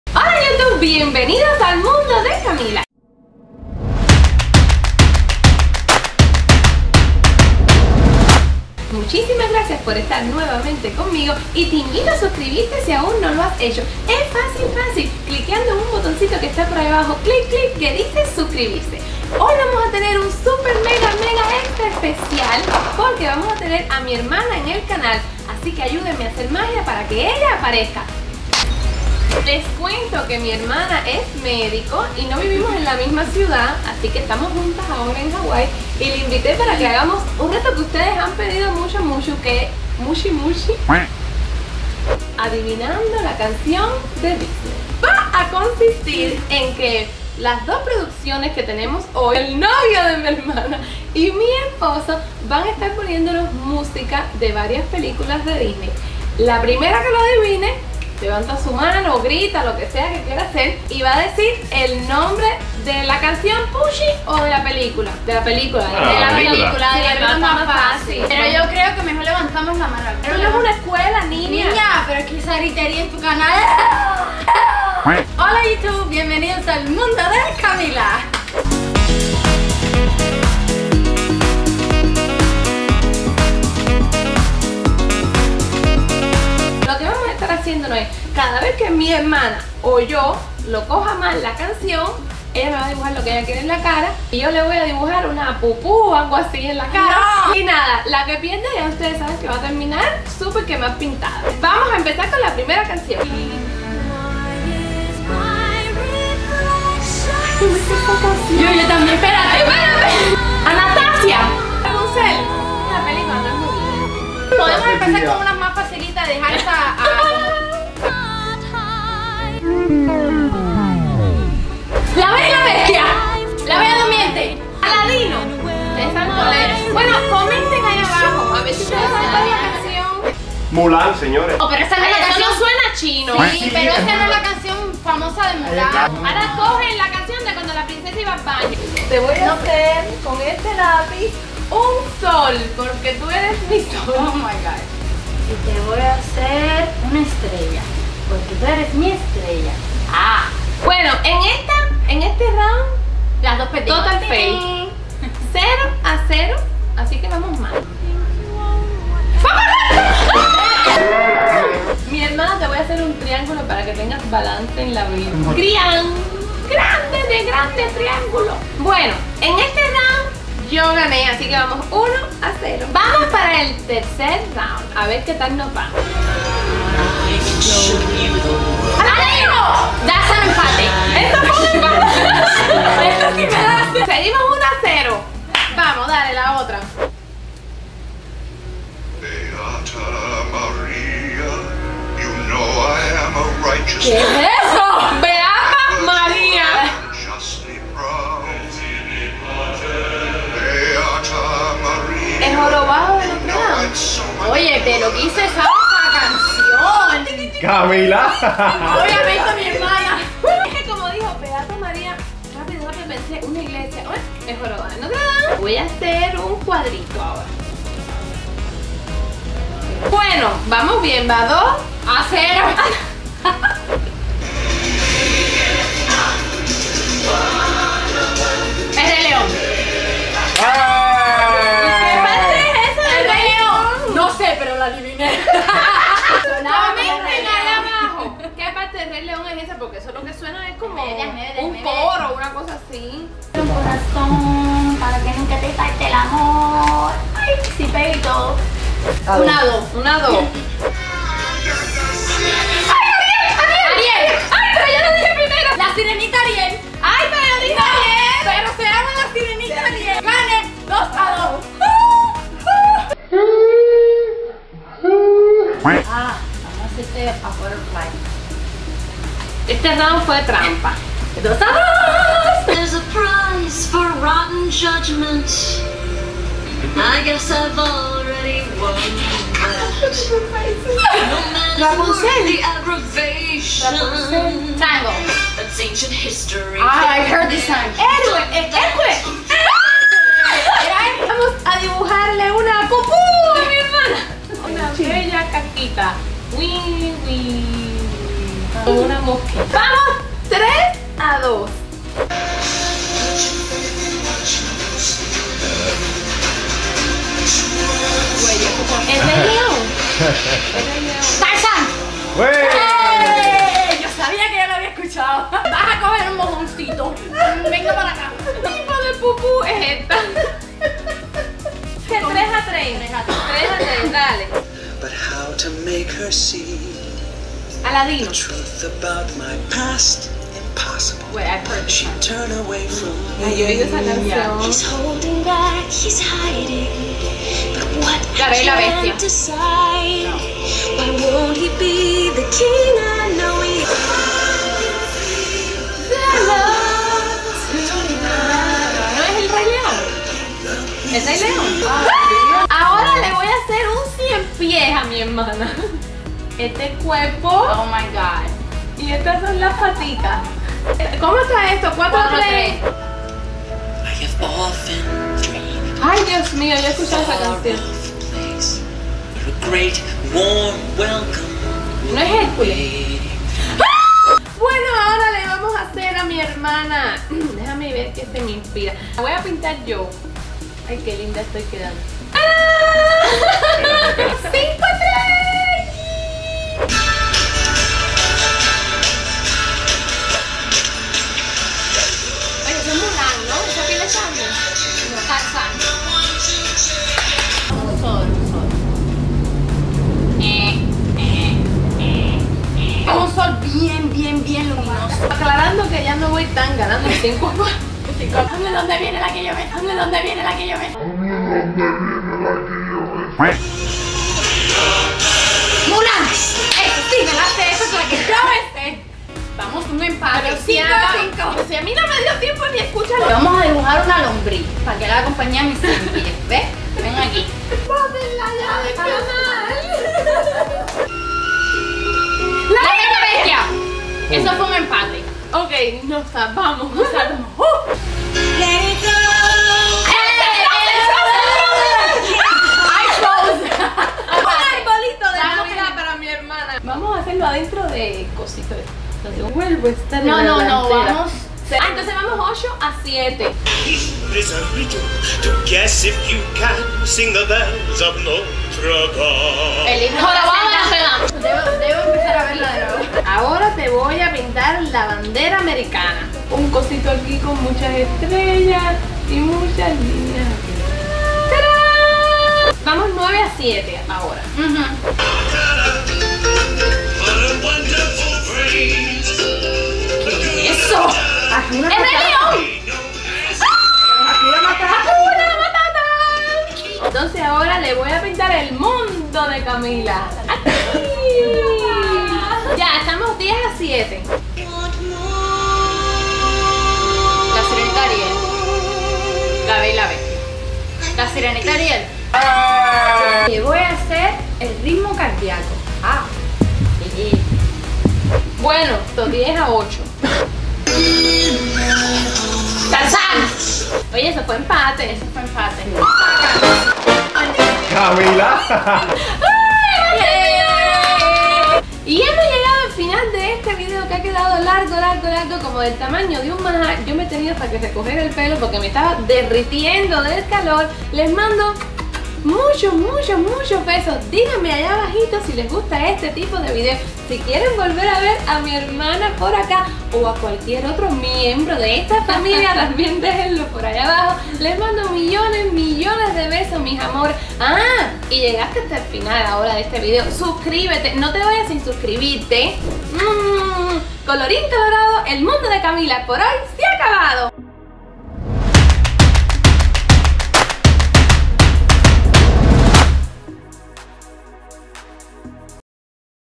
ADIVINANDO CANCIONES DE DISNEY - MANSION DE VACACIONES EN HAWAII - EL MUNDO DE CAMILA GUIRIBITEY
Hoy vengo con un reto muy divertido y les presento a mi hermana Patricia. Junto a ella compartimos y adivinamos famosas canciones del mundo magico de Disney, un mundo que todos de una manera u otra compartimos. Quien ustedes creen que jugó mas a las princesas cuando niña?

Mi hermana es super comica muchos like para este video si quieren que siga saliendo en mis videos :).

Para participar por el iPhone X:
1- Estar suscrito a mi canal
3- Comentar

 2018 latinas hermanas